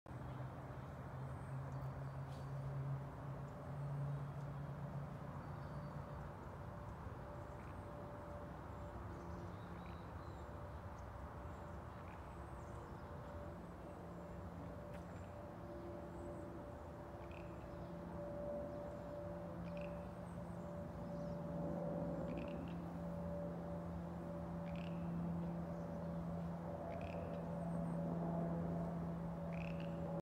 30 Seconds of Peace
The beautiful Upper Gardens, where St. Anne's Chapel, the oldest structure at The Grotto, is located.